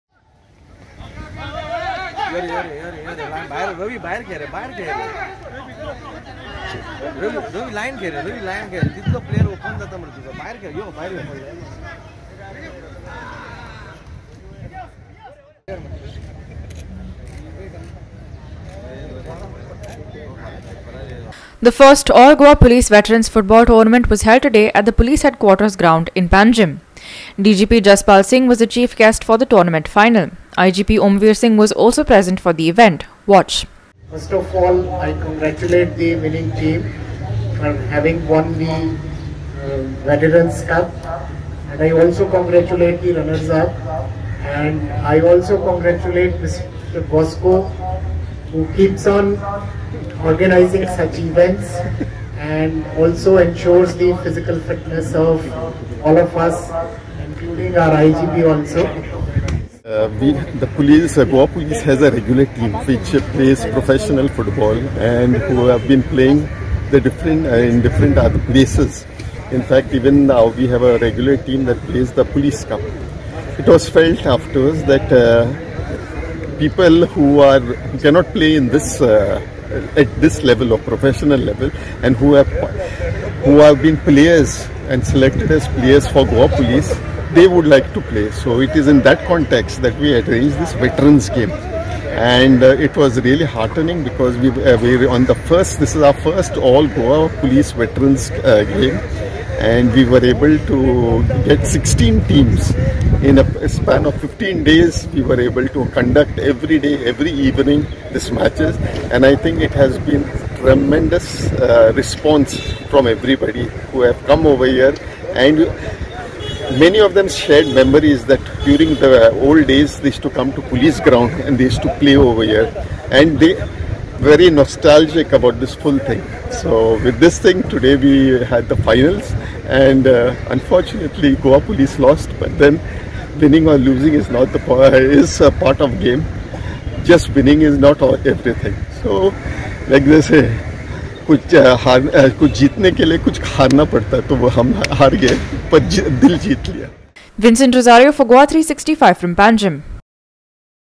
1st All Goa Police Veterans football tournament conclude today: WATCH
The 1st All Goa Police Veterans football tournament was held today at the police headquarters ground in Panjim. DGP Jaspal Singh was the chief guest for the tournament final. IGP Omvir Singh was also present for the event. Watch.